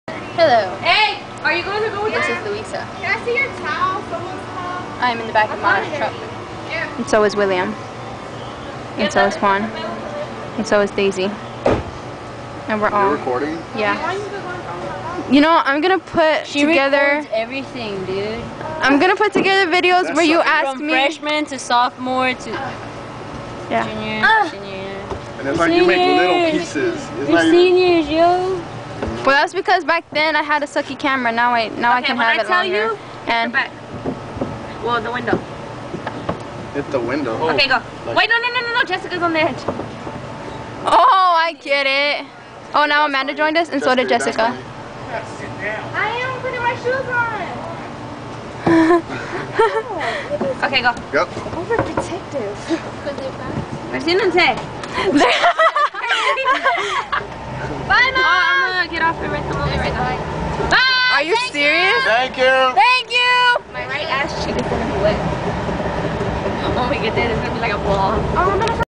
Mara's 17th B-Day Pool Party Part 6/10
Here We Are Leaving Jessica's House On Our Way To Ixamara's House To Watch Movies Riding In The Back Of The Her Mom's Truck